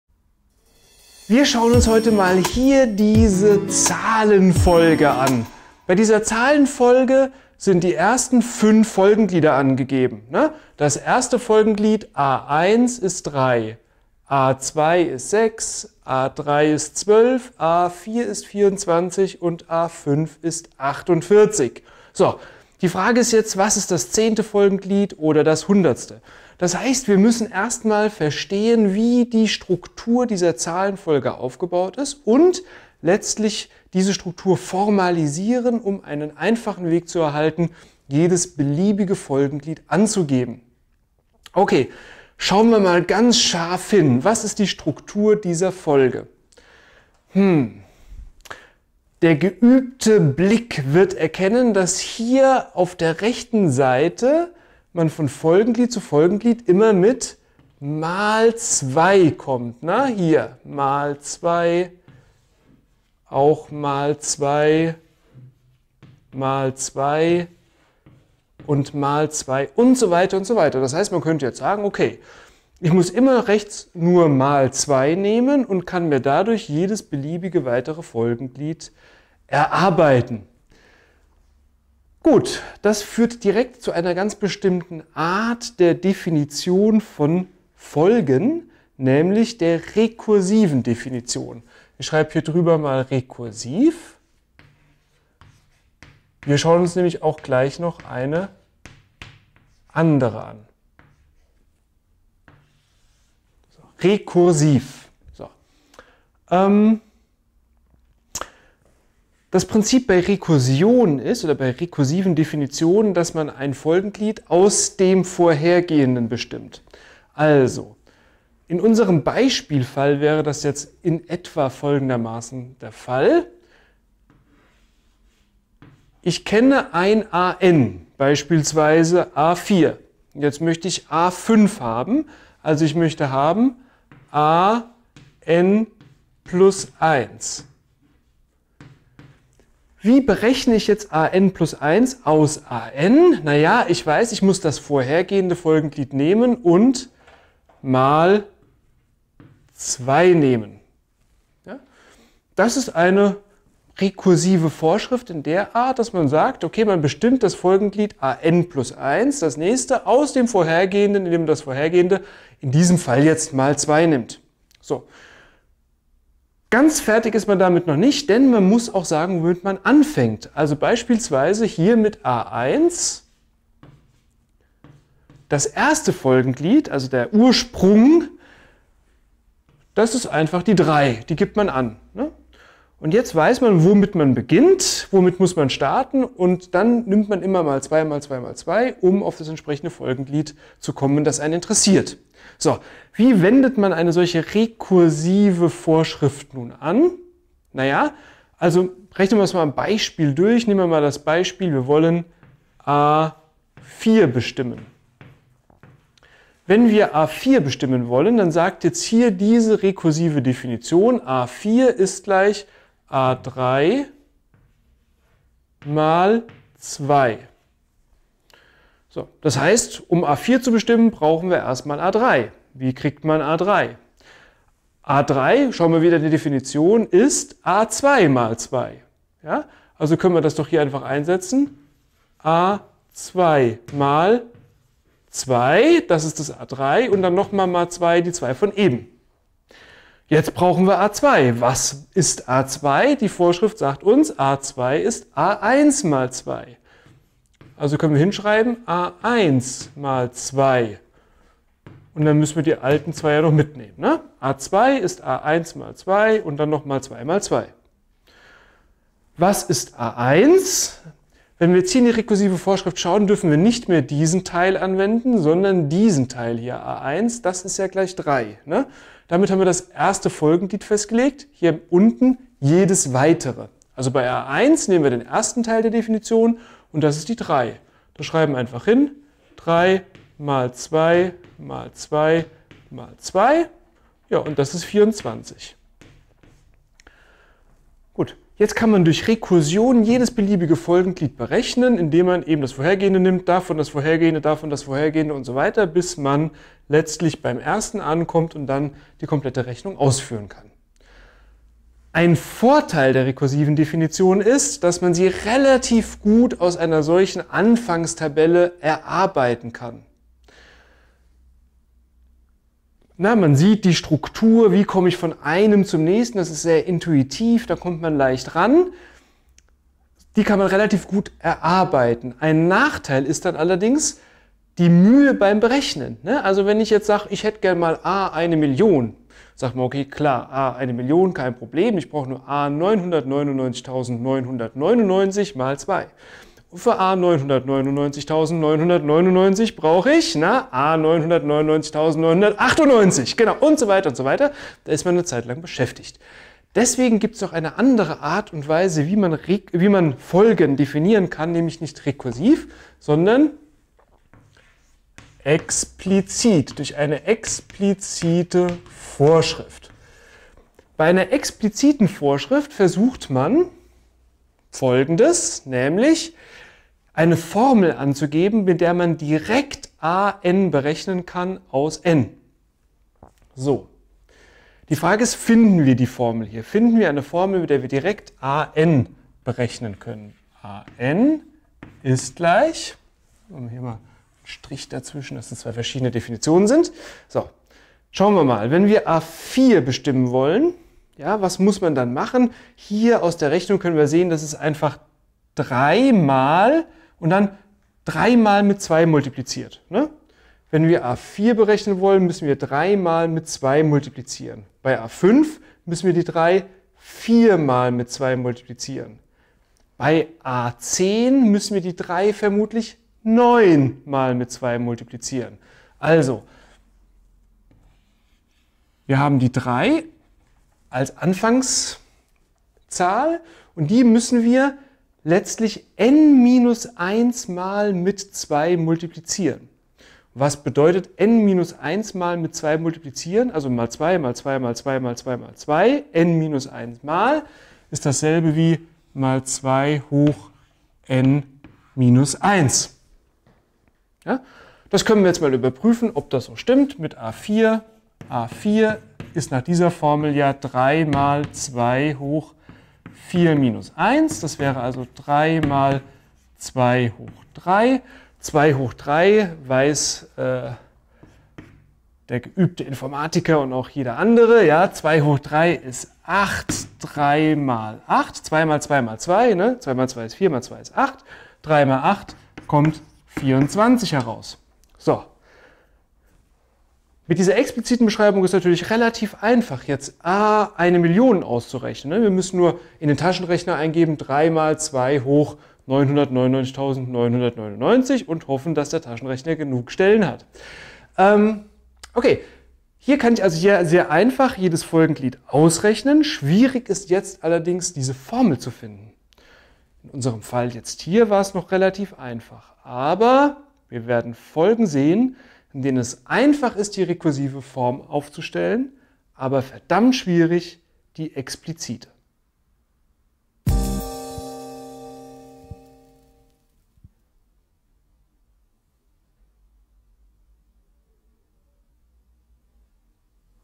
Der dunkle Lord und die Zahlenfolgen
🧑‍🏫Heutiges Thema: Explizite und Rekursive Zahlenfolgen
🎬Regelmäßige Livestreams, auch direkt aus dem Hörsaal!